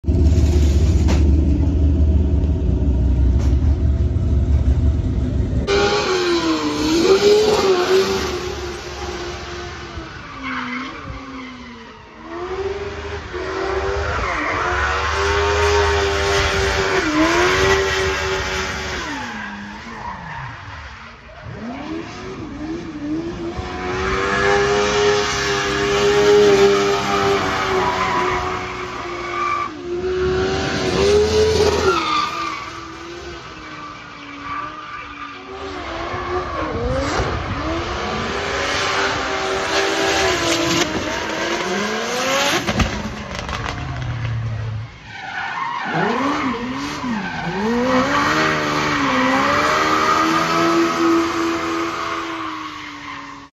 🥉(Pro2 Category) Jérôme Arribert - BMW with dual Gixxer1000 at Drift Kings 2023 Round 3 France 🇫🇷
🥉(Pro2 Category) Jérôme Arribert - BMW E36 compact with 2x Suzuki GSX-R 1000 drifting at Drift Kings International Series 2023 Round 3 DRIFT CUP MSO at Circuit de l'Anneau du Rhin🇫🇷

🎥: Máté Nuszer